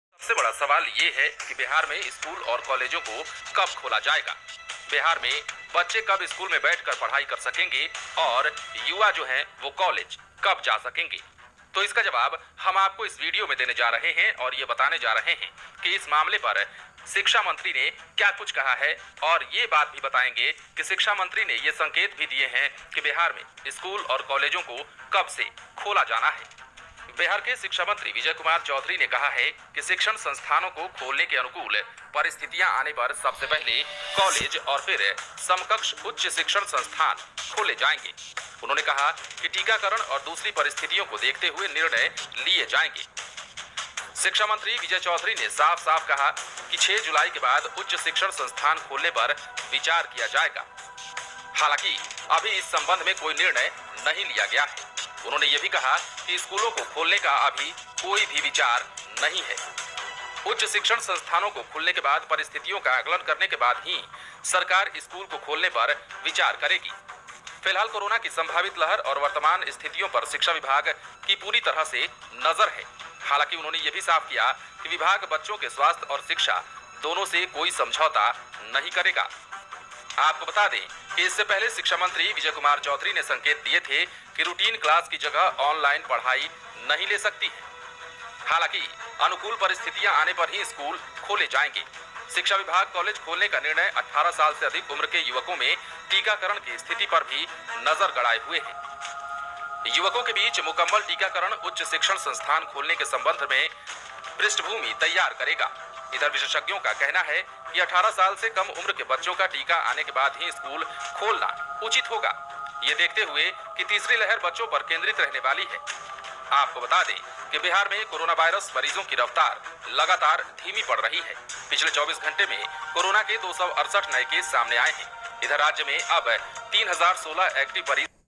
ITI NEWS TODAY || आईटीआई कॉलेज कब खुलेगी, एग्जाम कब से होगा, आइए सुनते शिक्षा मंत्री ने क्या कहा
# iti today news#itiexam